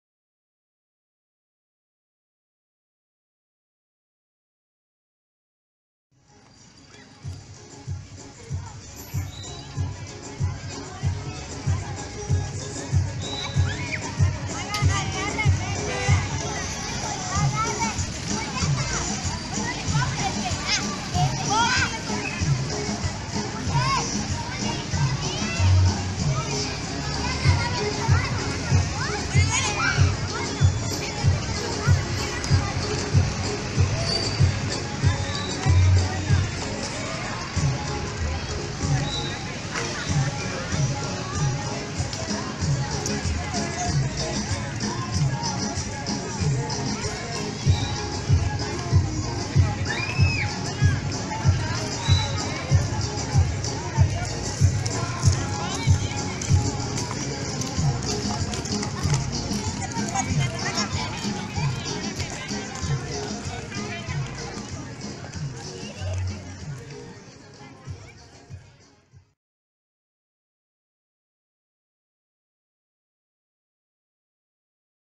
6-Agita Pilar 5ta. Edición 2018 - FCA-UNP
Con el lema “Sea activo todos los días" Agita Pilar, en su quinta edición mostró nuevamente a la ciudadanía pilarense las diversas maneras de hacer actividad física todos los días. Con la presencia de más de 800 personas en la plaza Centenario del Barrio San Antonio de la Ciudad de Pilar, el Instituto Superior de Educación Física, de la Facultad de Ciencias Aplicadas de la UNP, a través del programa Universidad en Movimiento ha ofrecido diversas actividades físico - recreativas para niños jóvenes, adultos y adultos mayores.